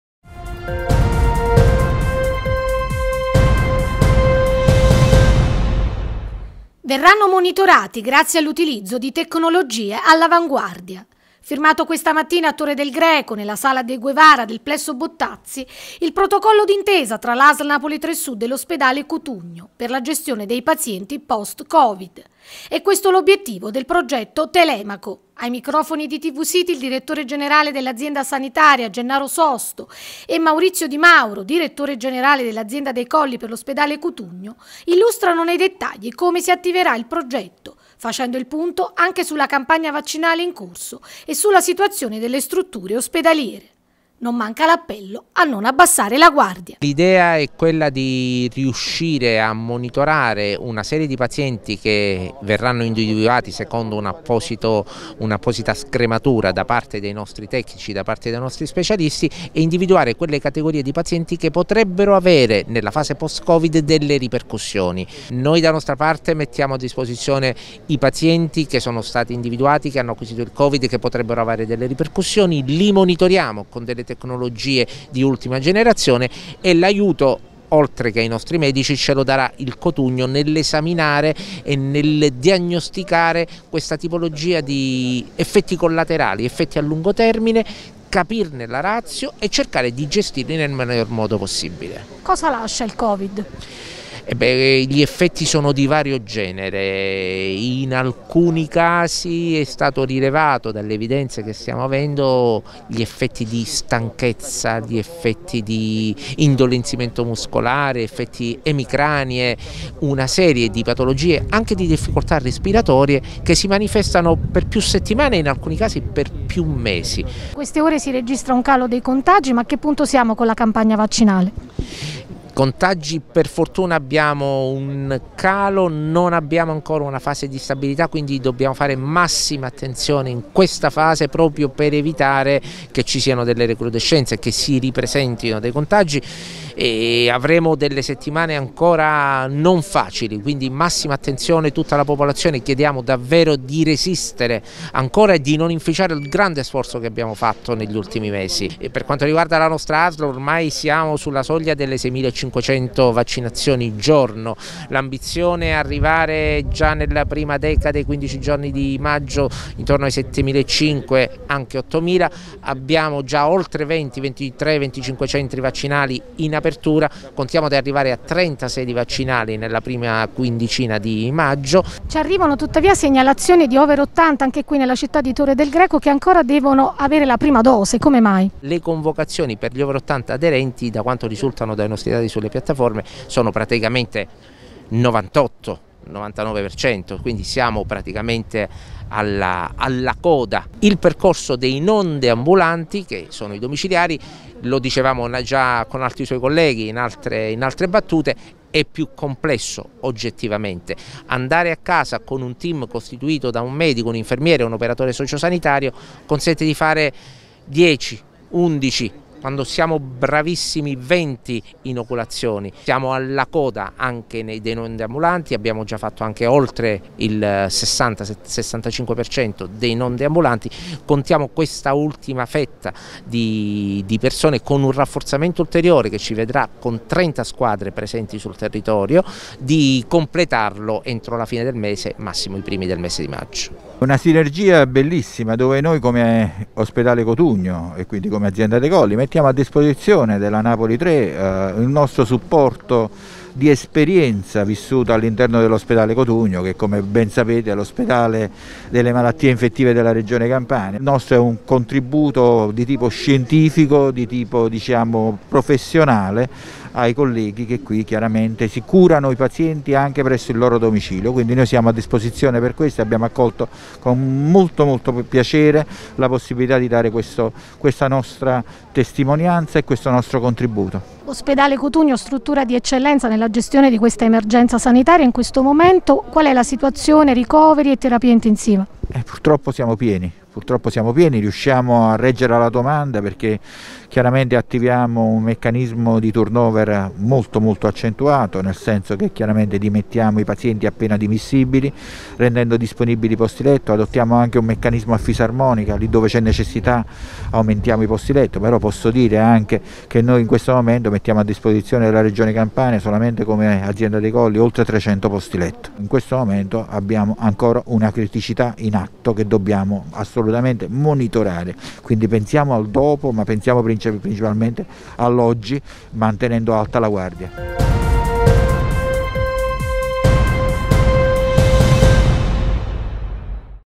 Gestione pazienti post-covid: intesa tra AslNa3Sud ed ospedale Cotugno
Verranno monitorati grazie all’utilizzo di tecnologie all’avanguardia. Firmato questa mattina a Torre del Greco nella sala De Guevara del plesso Bottazzi il protocollo d’intesa tra l’Asl Napoli3sud e l’ospedale Cotugno per la gestione dei pazienti post Covid.

E’ questo l’obiettivo del progetto Telecamo. Ai microfoni di TVCITY il direttore generale dell’azienda sanitaria Gennaro Sosto e Maurizio Di Mauro, direttore generale dell’Azienda dei Colli per l’ospedale Cotugno illustrano nei dettagli come si attiverà il progetto facendo il punto anche sulla campagna vaccinale in corso e sulla situazione delle strutture ospedaliere. Non manca l’appello a non abbassare la guardia.